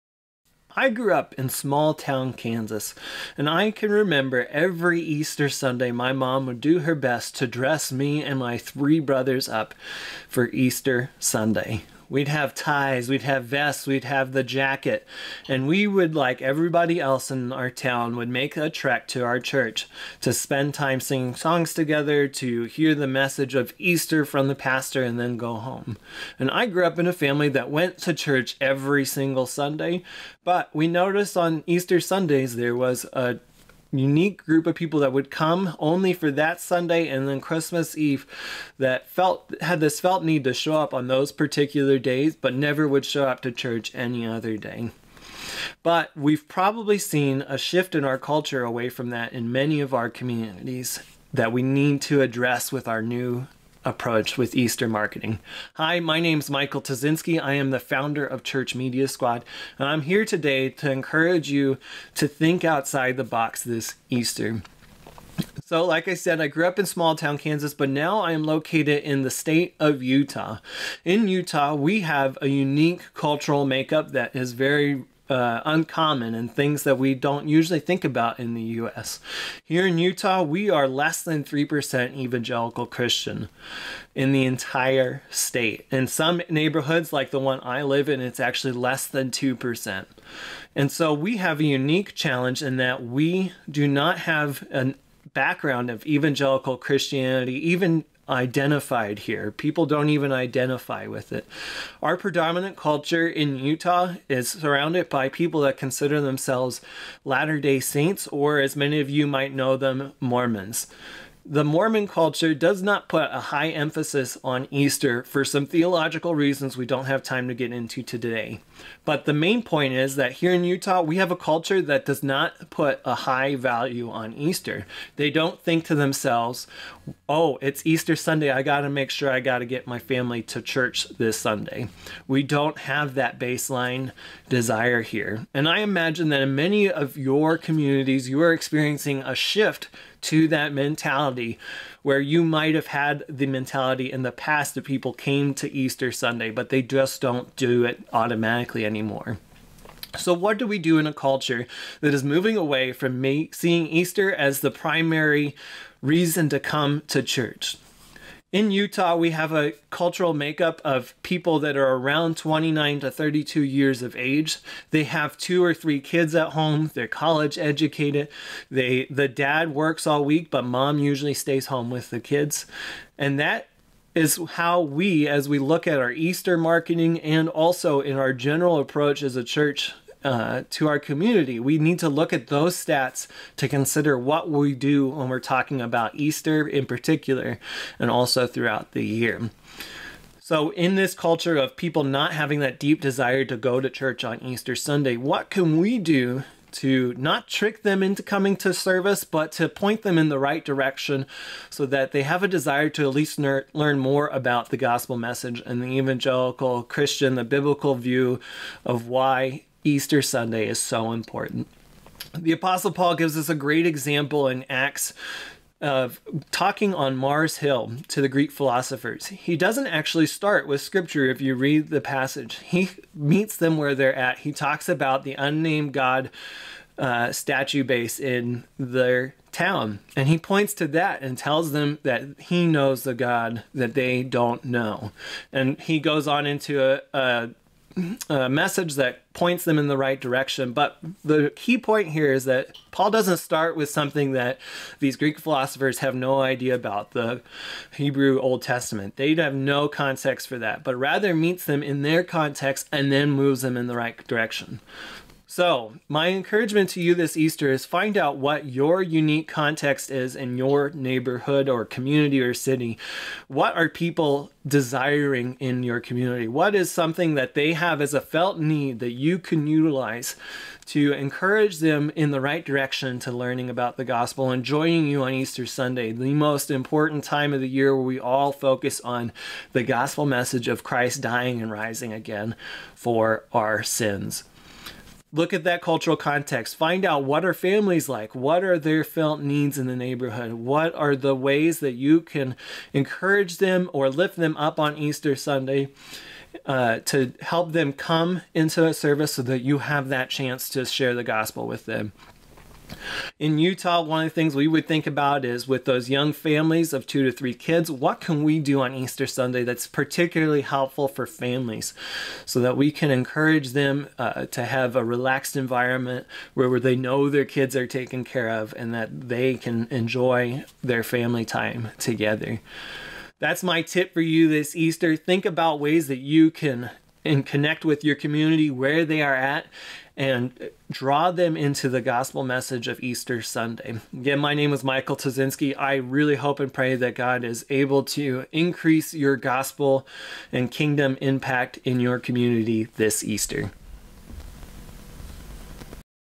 Easter Tips from Michael Tuszynski
Michael encourages you to think differently when it comes to marketing to those who do not see the importance of church.